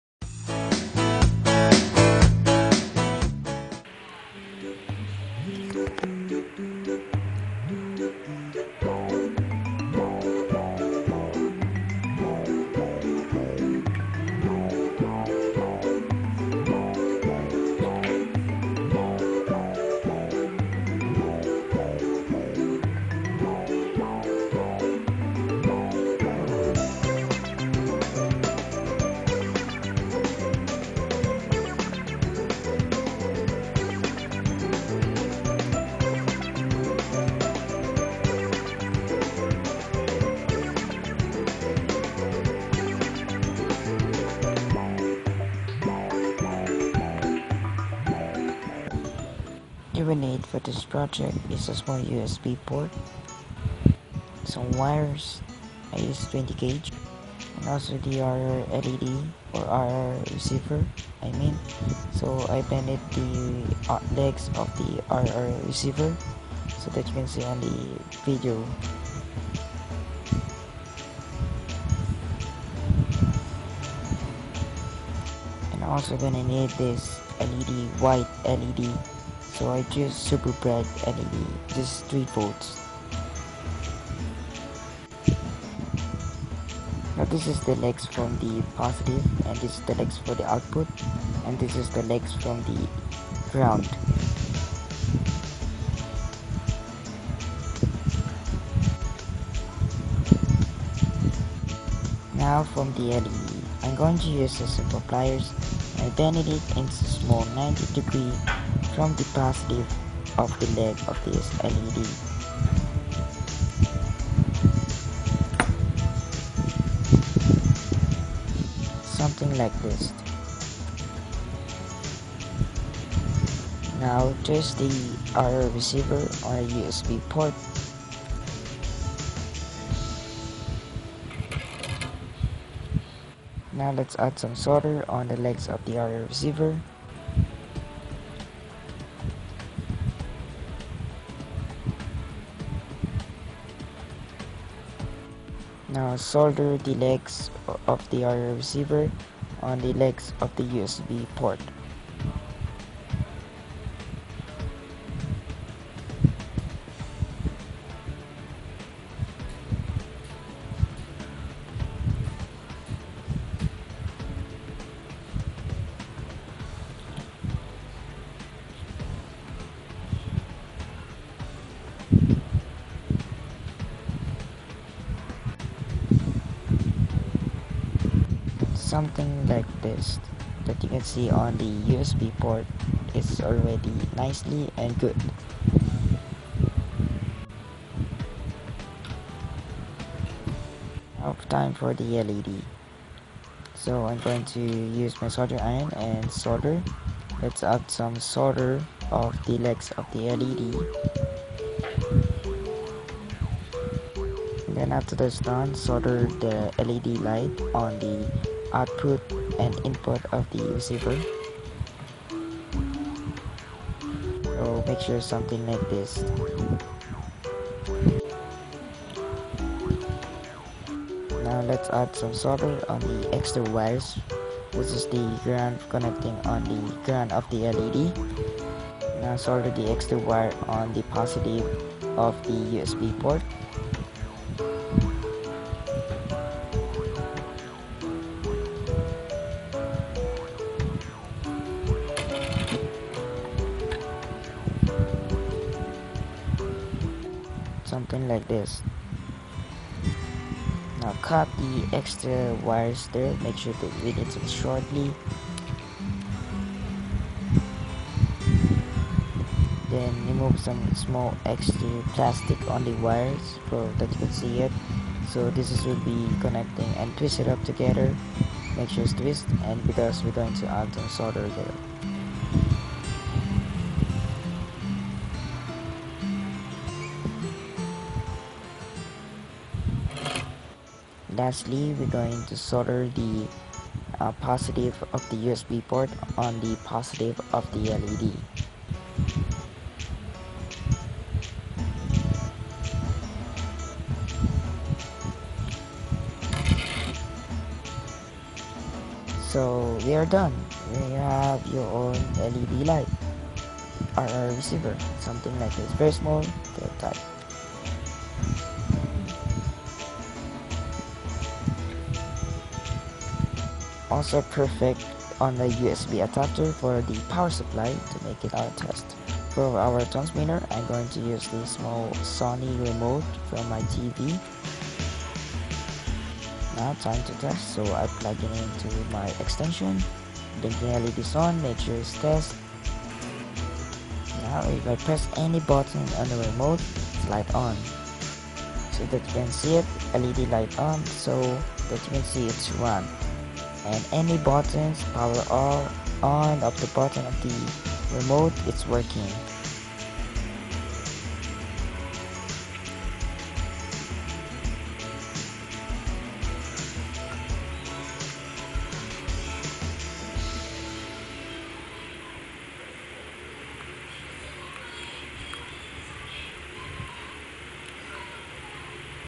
How to make your own USB ir led light!
This is step by step to how to make your own ir led light